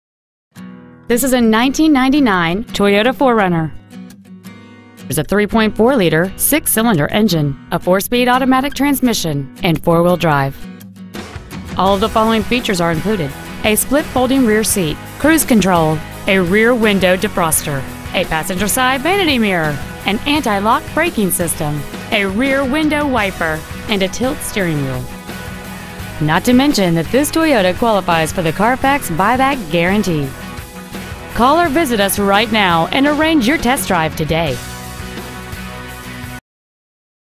1999 Toyota 4Runner Denver CO 80221
Year : 1999
 Make : Toyota
 Model : 4Runner
 Engine : 3.4L V-6 cyl
 Trans . : 4-Speed Automatic
 Exterior : Millennium Silver
 Miles : 156,539
 Interior : Gray
 Mountain States Toyota
 201 W. 70th Ave.
 Denver , CO 80221
 Pre-Owned 1999 Toyota 4Runner Denver CO